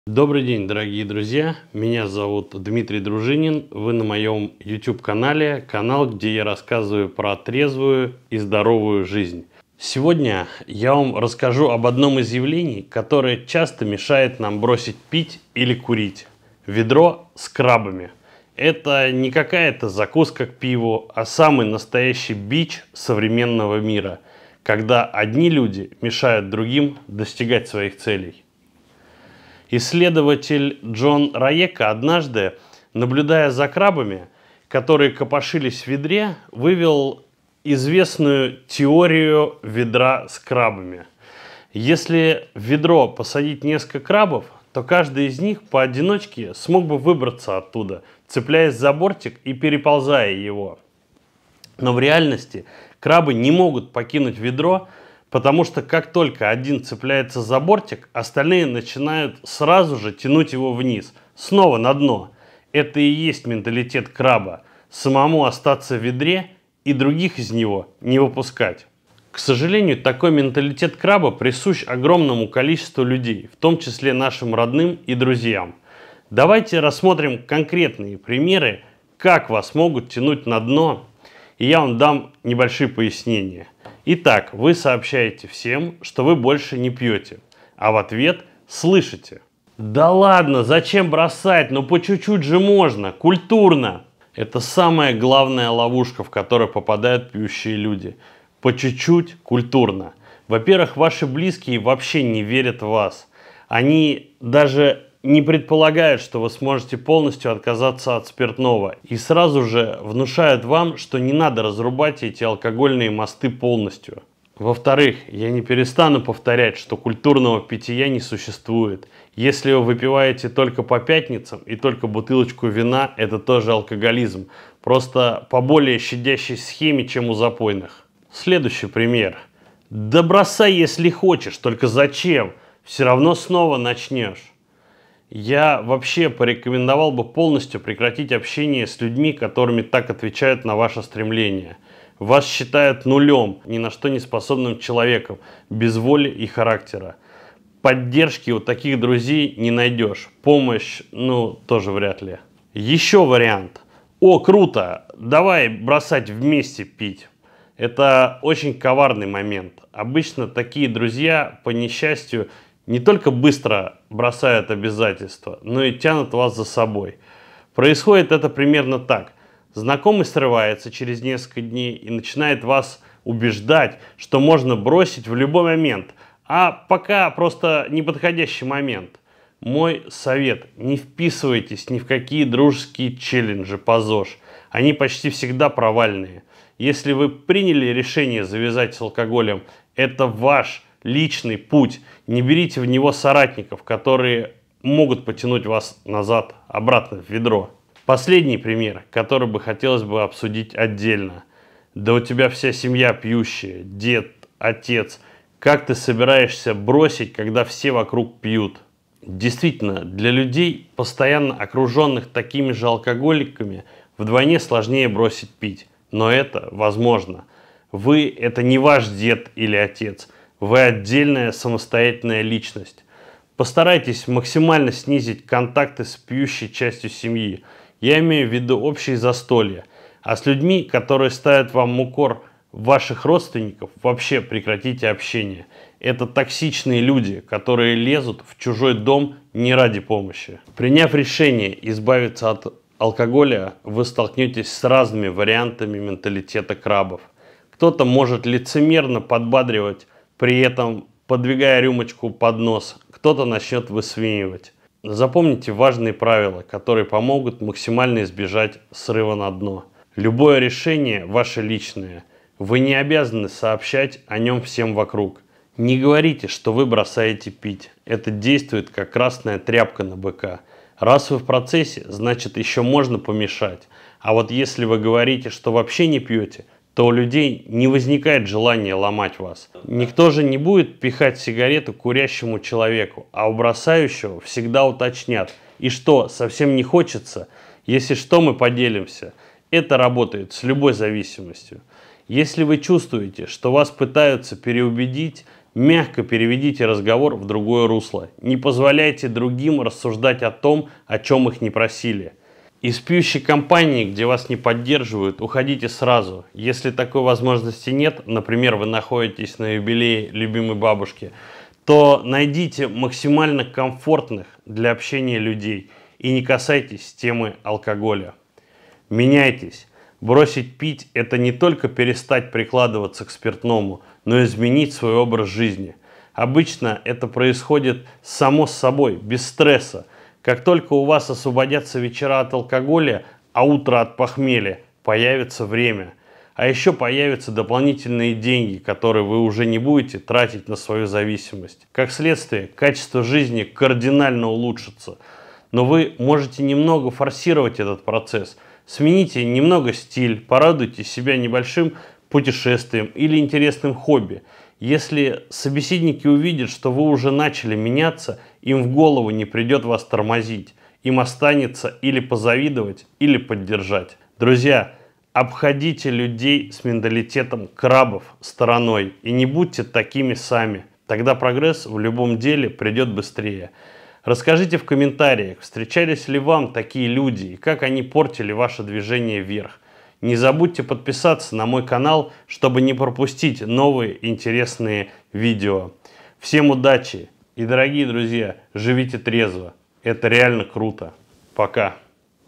Принцип ведра с крабами
Сегодня мы поговорим о принципе ведра с крабами. Как он мешает нам избавиться от зависимости.

Там вы найдете много интересной и полезной информации. Давайте житьтрезво вместе.